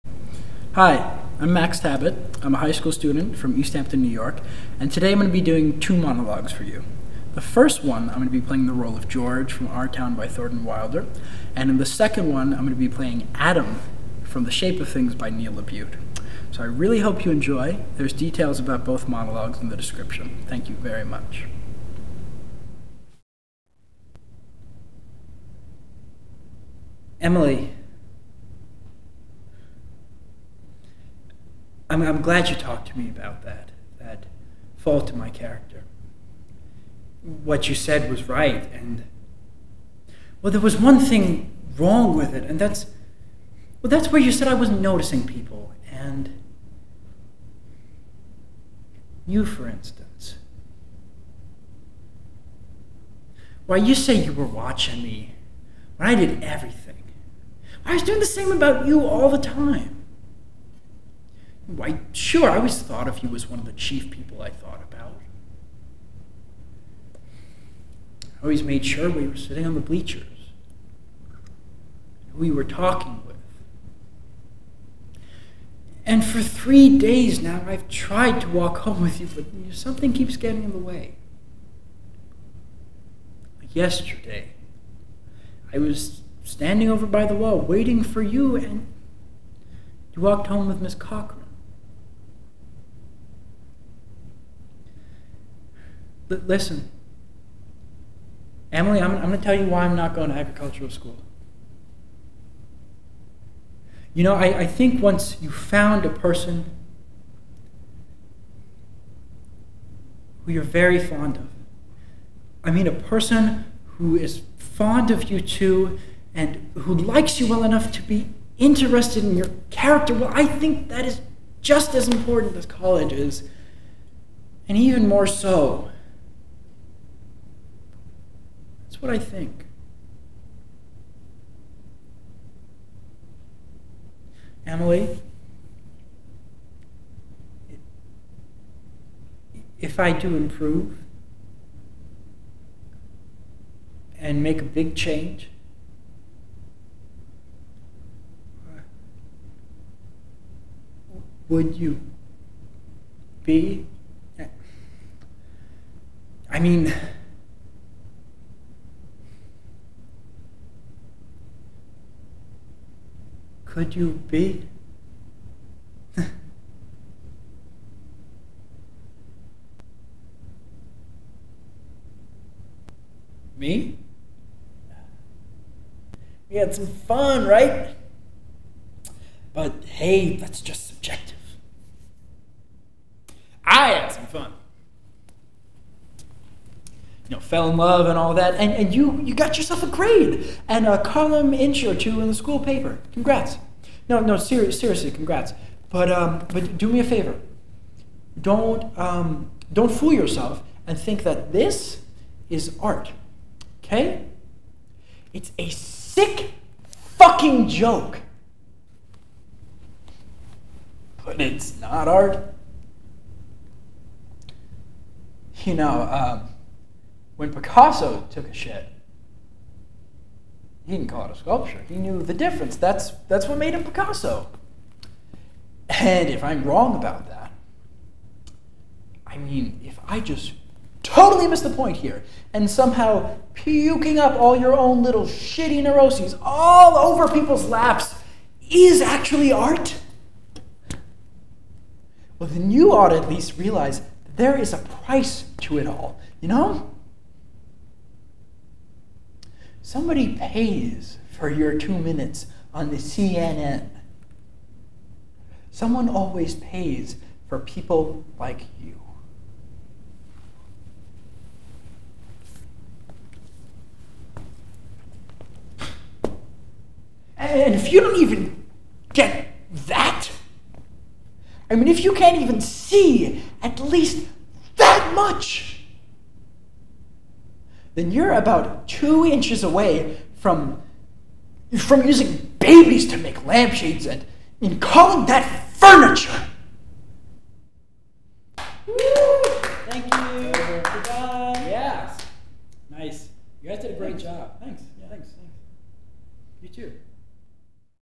Maximilian Tabet College Arts Supplement
Maximilian Tabet's Theater Art Supplement

Monologue #1 (George, from Our Town): As they walk home together, Emily remarks that a change has come over George since he became a local baseball star. She says he has become "conceited and stuck-up." Although hurt, George takes her words to heart. Emily, suddenly mortified at her own bluntness, apologizes to George and begins to cry. George tells Emily not to be concerned and invites her to have an ice-cream soda with him at the local drugstore. Emily and George sit at the counter and talk about the future. George talks about his tentative plans to go to the State Agriculture School. Throughout the conversation, however, George weighs the idea of continuing his formal education against the idea of staying in Grover's Corners with Emily, revealing his fondness for her.

Monologue #2 (Adam, from The Shape of Things): Evelyn has just torn Adam's world apart by revealing that he is her Senior Art Project. She spent months facilitating his changes in both attitude and appearance, and is now holding his old clothes, the engagement ring he gave to her, and videos of their love-making on display in a gallery. Appalled by her actions, Adam confronts her in order to validate himself, and to define the line between "art" and absurdity. Shockingly, Evelyn asks him if he has any "hard feelings."